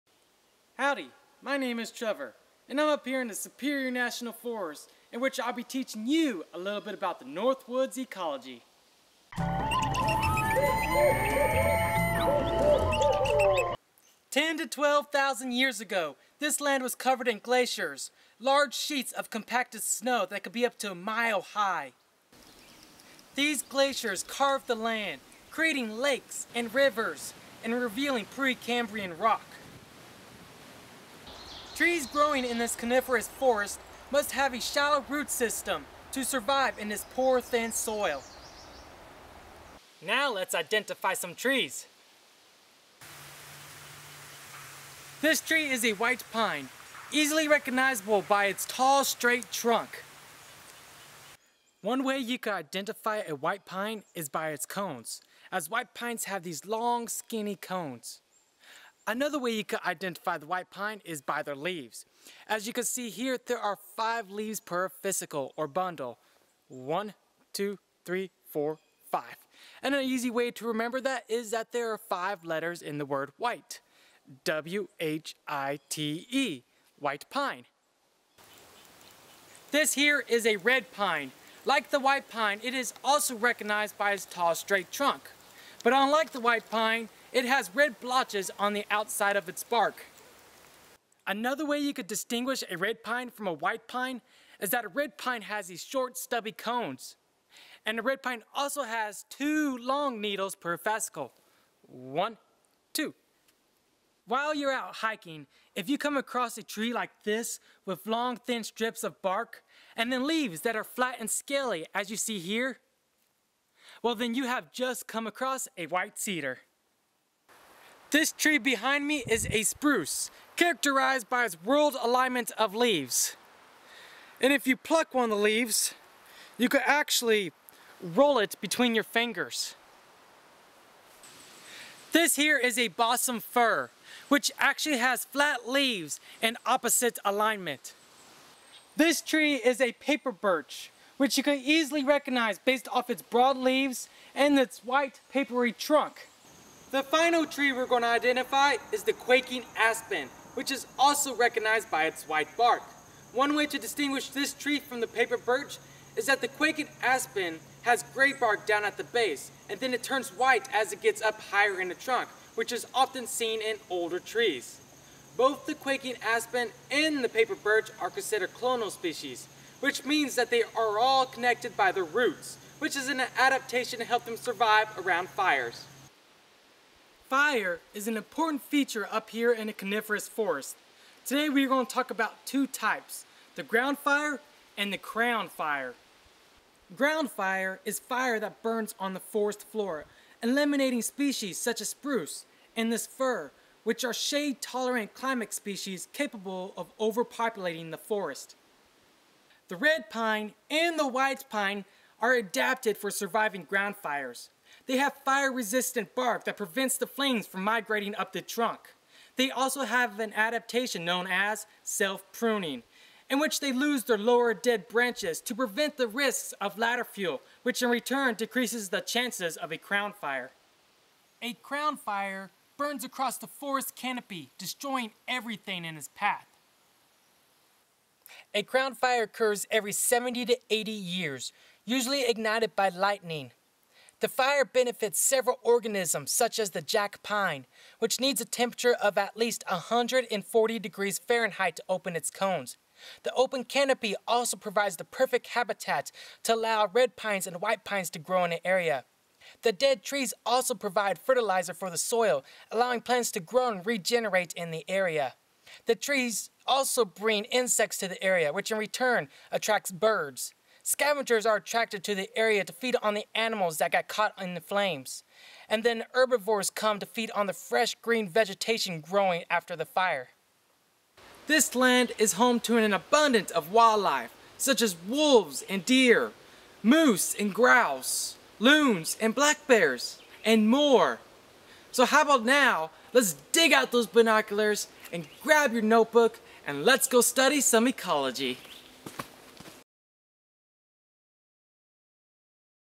Northwoods Ecology | The Natural History of Northern Minnesota
Do you want to learn about some of Northwoods Ecology, then you have come to the right place. Learn how to identify trees in Northern Minnesota.  How was the landscape formed? Check out this video to find out how.